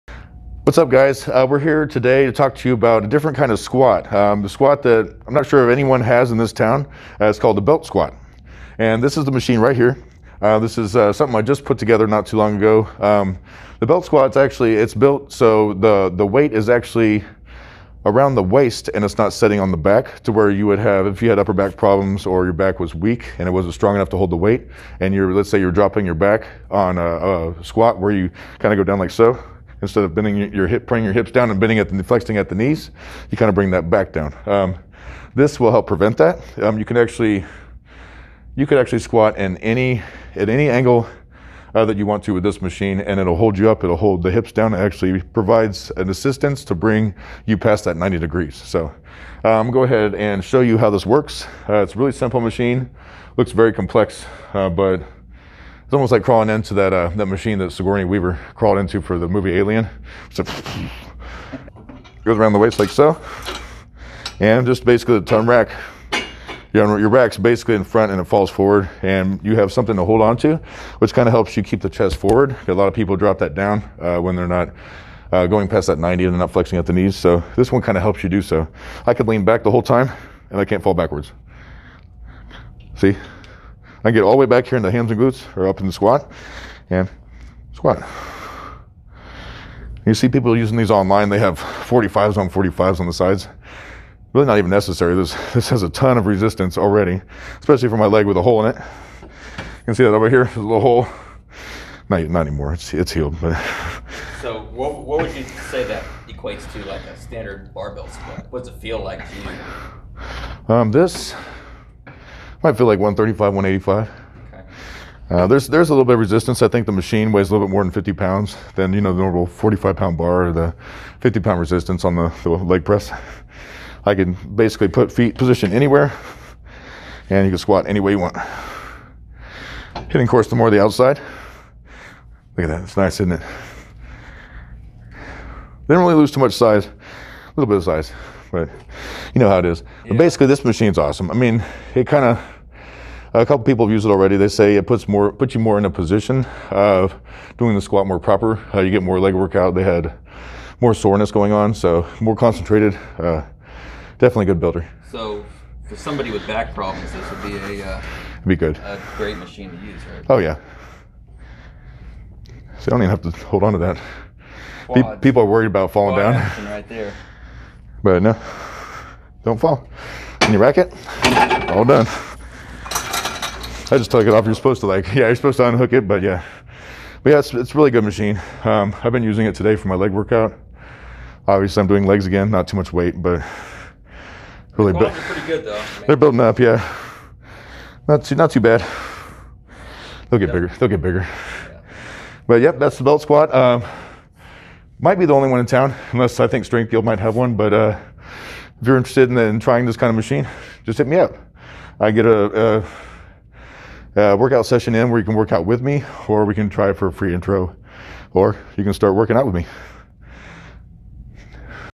Belt Squat Machine...A game changer in the leg training game!
The belt squat machine is a game changer in the leg training game! With the weight being distributed around the waist rather than on the shoulders, it becomes a method of choice for those that are either at the beginning level or those that have back issues. No compression of the spine as with the standard barbell squat. REPS FITNESS is one of the few places in the area that has this beautiful piece of leg training bliss!